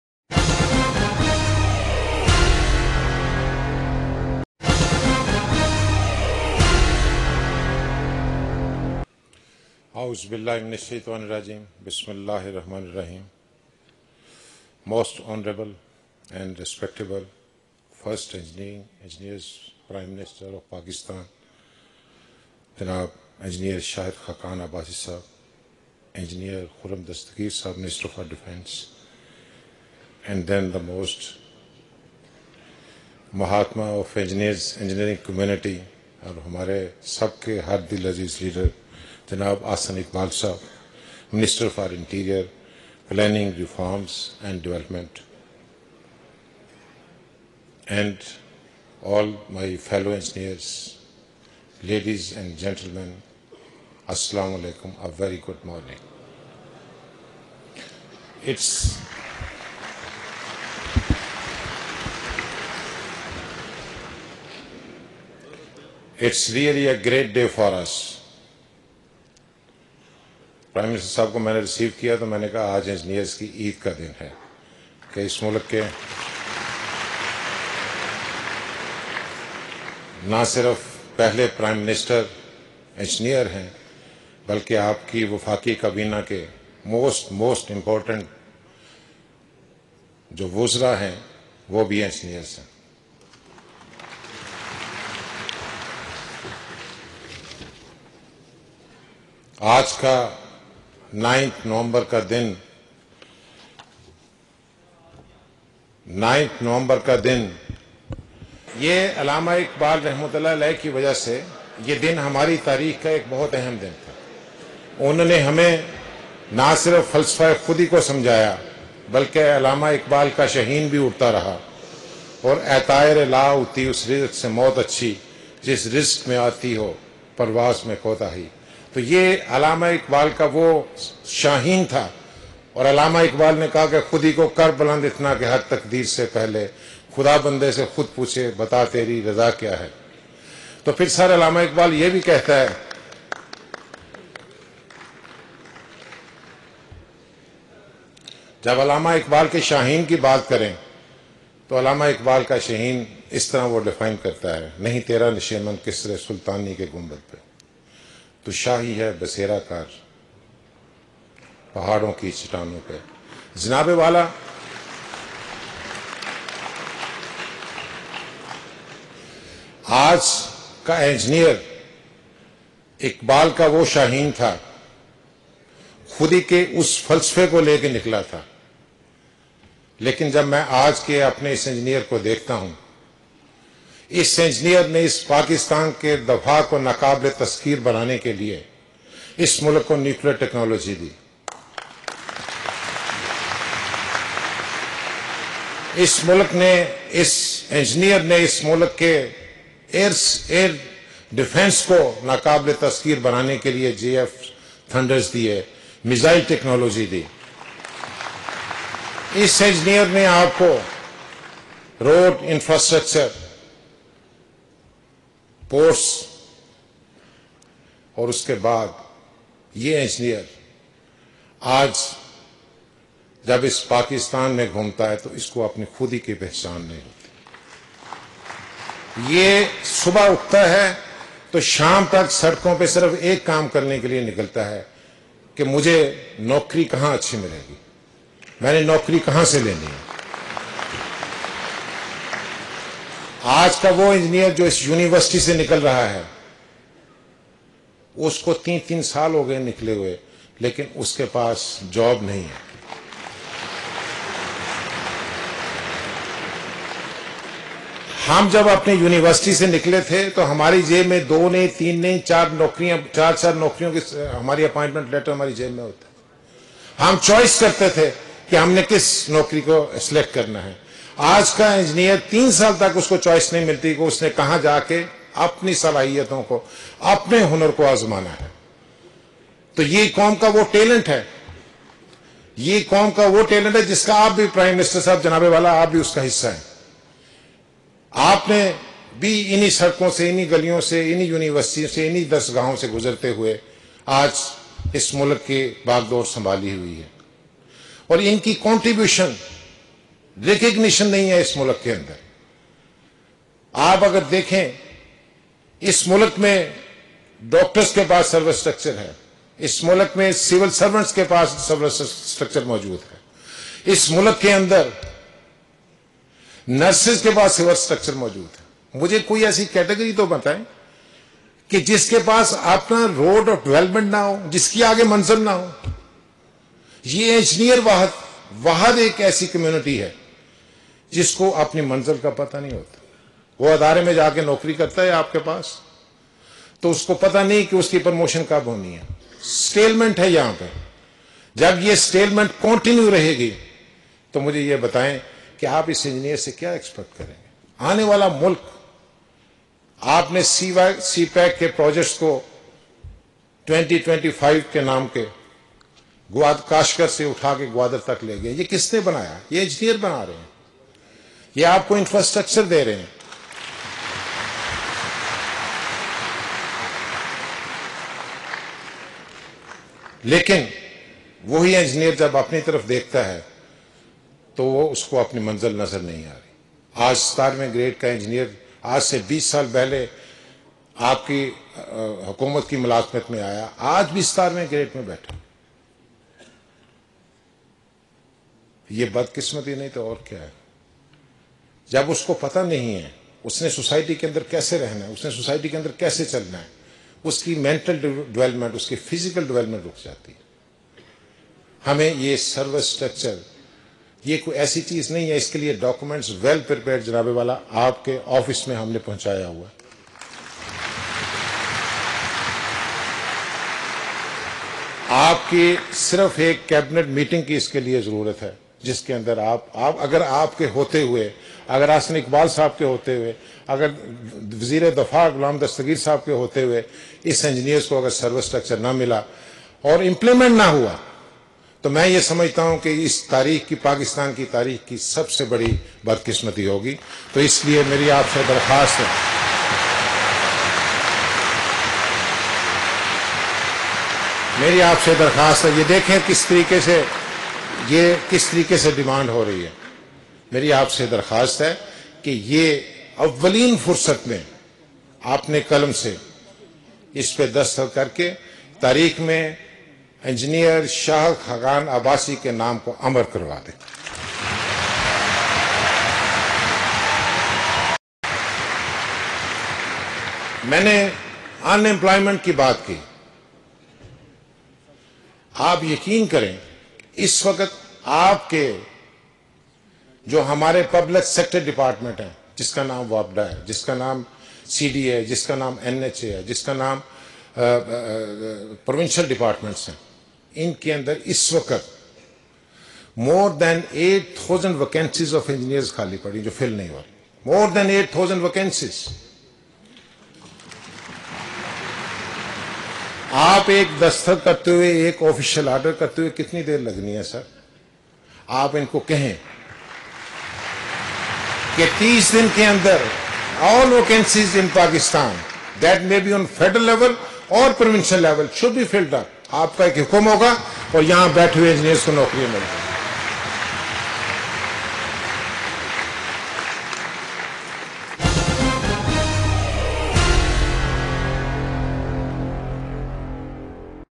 Engineering Convention, Speech of PEC Chairman Part 1
Engineer Jawed Salim Qureshi Speech 9 Nov 2017